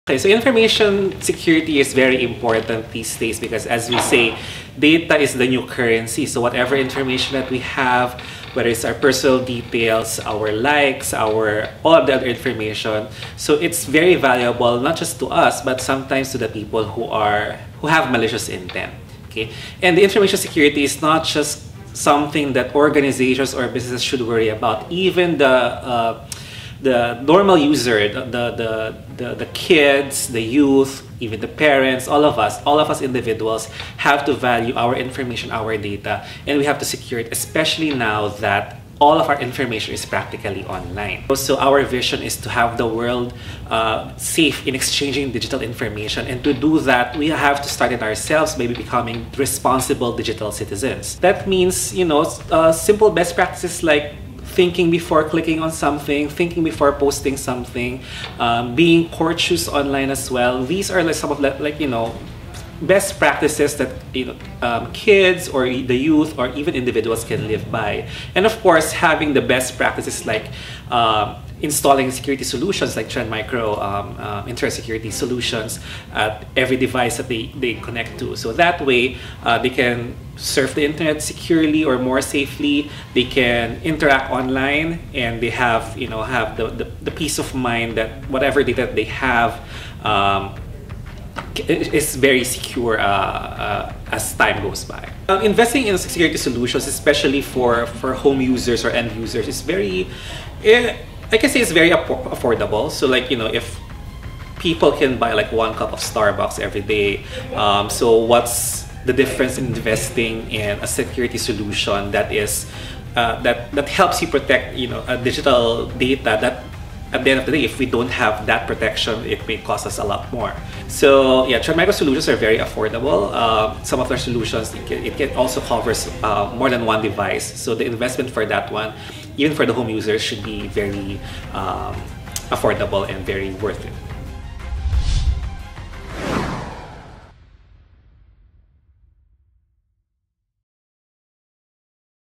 How to Protect your Privacy and Data in the Cyber World
Investing in Cyber Security Solutions is like buying a cup of coffee. If you can afford to buy a cup of coffee you must be able to see the value of investing on your safety online away from the cyber predators .